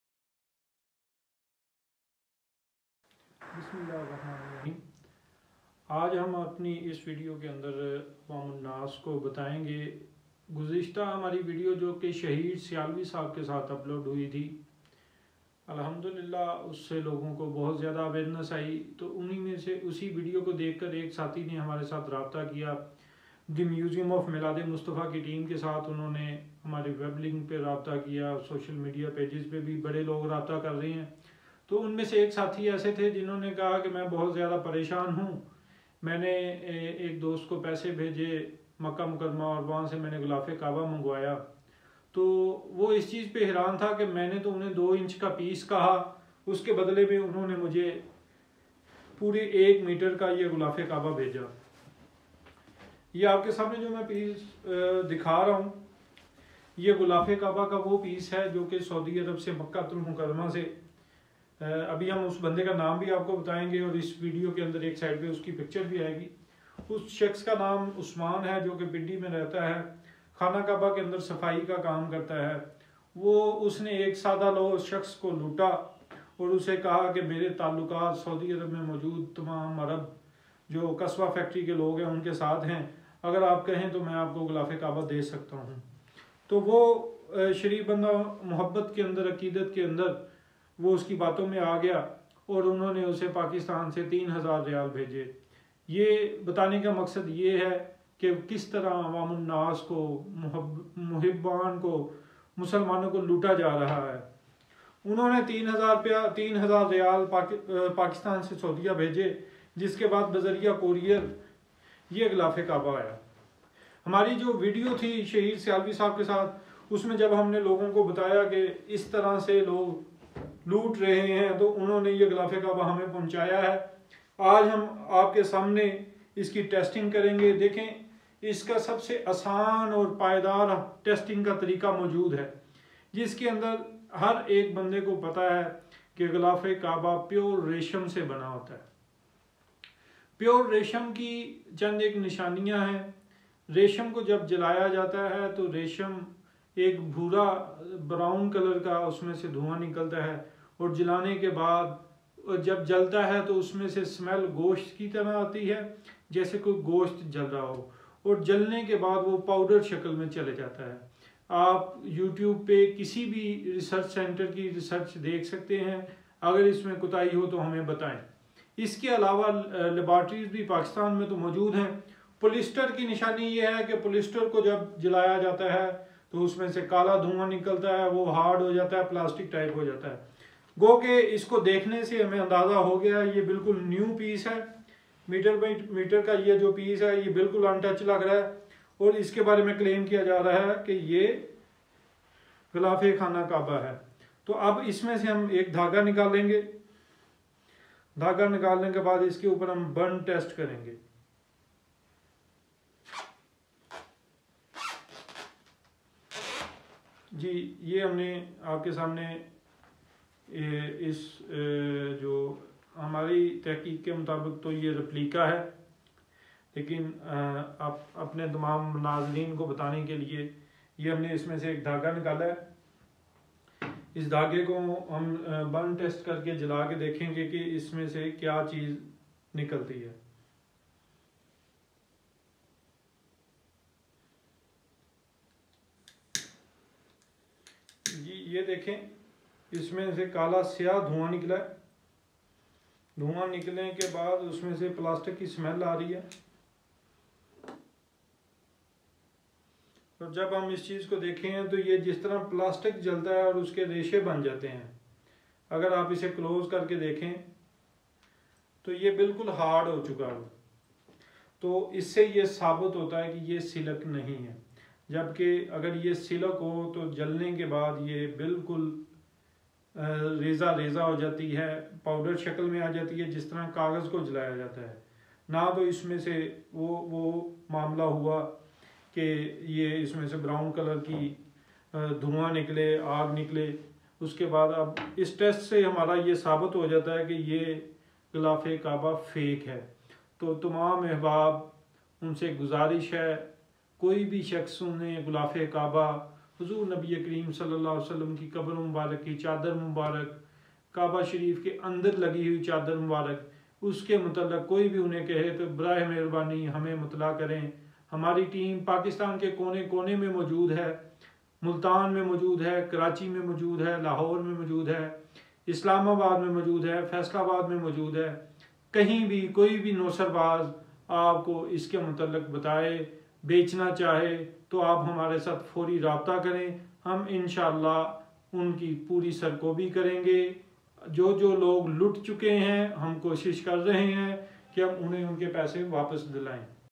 Expose fake Gulaf e kaba sale in Pakistan
پاکستان میں بڑھتے ہے جعلی تبرکات کے وار ۔ نوسر باز افراد نے مُقدس ہستیوں اور مُقدس مقامات کو بھی نہ بخشا۔